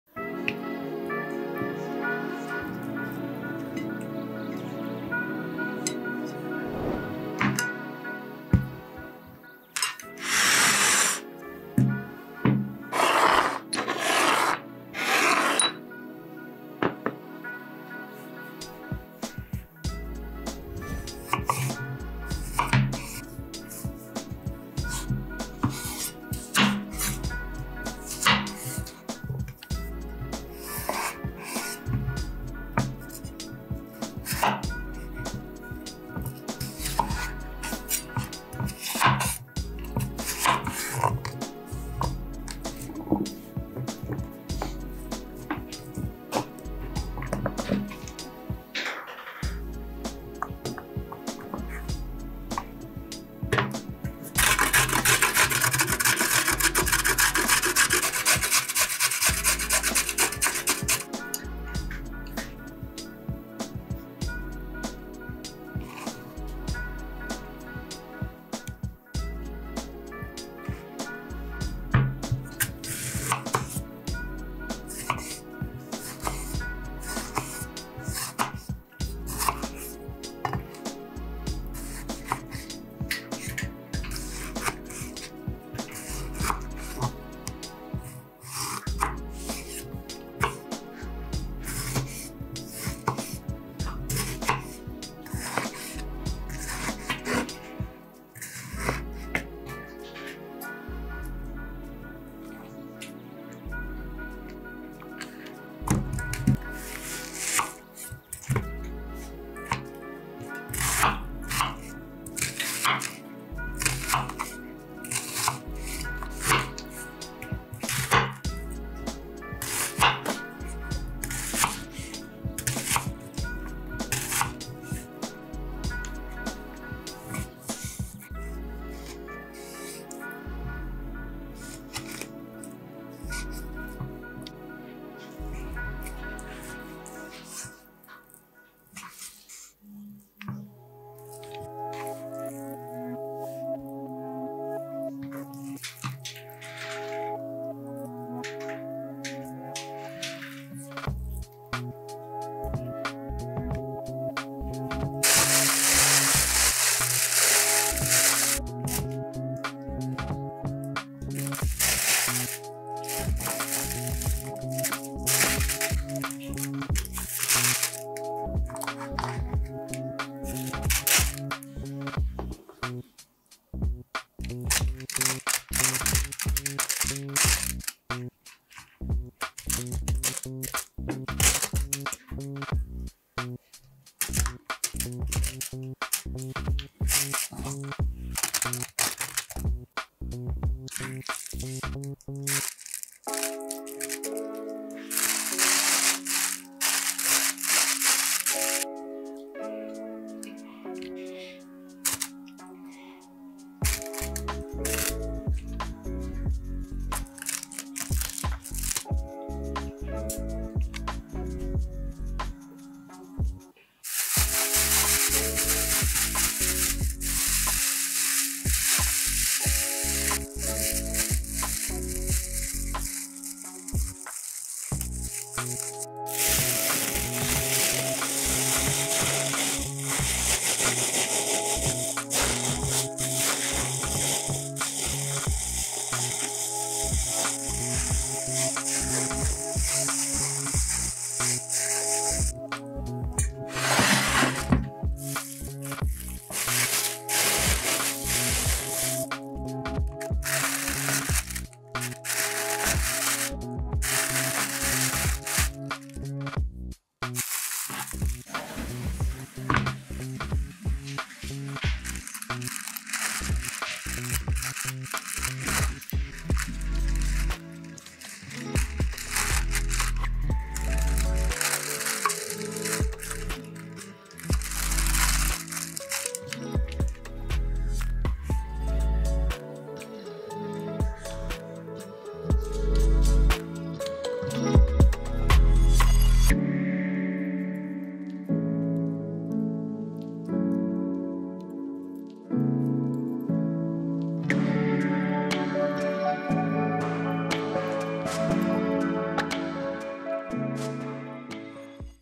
So lecker, dass ich jede Woche koche! Alles in der Tasche und im Ofen! Fast Dinner Rezept #64
So lecker, dass ich jede Woche koche! Ich habe einfach alles in eine Tüte und in den Ofen gelegt! Ein schnelles Rezept für ein Abendessen aus einfachen und billigen Produkten. Schnell kochen, lecker kochen! Ein schnelles Rezept zum Mittag- oder Abendessen.
ZUTATEN:
- Schweinefleisch - 1 kg, Kartoffeln - 1 kg, Tomate, Zwiebel, Karotte, Pflanzenöl 50 g, Gewürze für Fleisch, Hopfen - Suneli, Salz.
REZEPT :
- Kartoffeln würfeln, Karotten auf einer groben Reibe reiben, Tomate hacken, Zwiebel grob hacken, Fleisch grob hacken
- Alles in Schichten in einen Backbeutel legen.
- 60 Minuten bei 180 ° C backen

 Guten Appetit!!!
IMMER Untertitel einschließen!